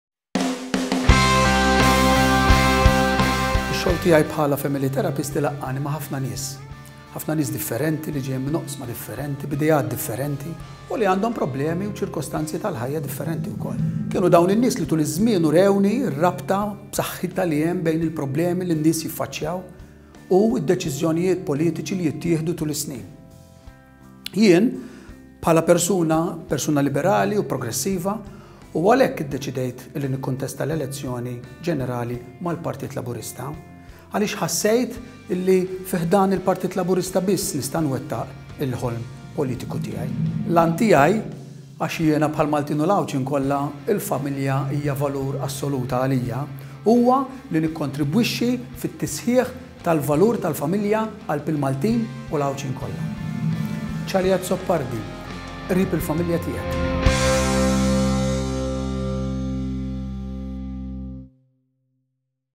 Charlie Azzopardi Family Therapist Partit Laburista.mov
Dr. Charlie Azzopardi talks about why he has chosen to contest general elections